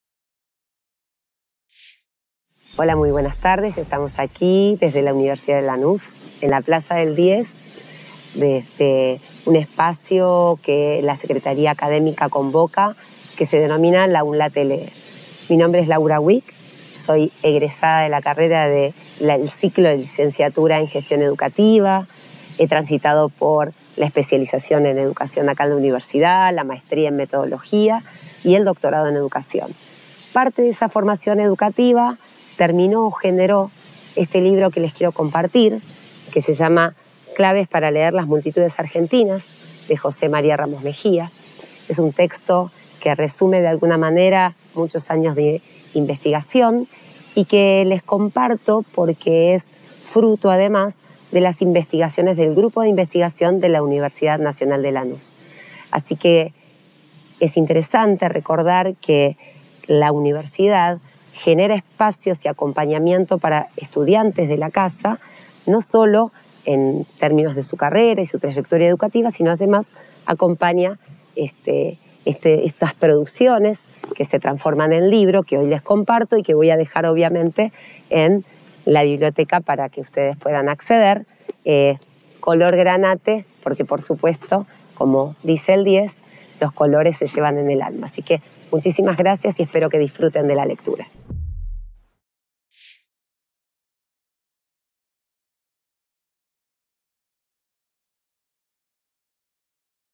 Laura Guic - La UNLa te lee📖 Edición 2022🔥
📚Hoy en la UNLa te lee nos acompaña Laura Guic y nos recomienda “Claves para leer las multitudes argentinas de José Ramos Mejía”, un libro de su autoría, producto de investigaciones que se realizaron en la UNLa.

✒️Laura es egresada de la UNLa y docente investigadora del área de Historia del Departamento de Humanidades y Artes.

Edición 2022 - Temporada 3🔥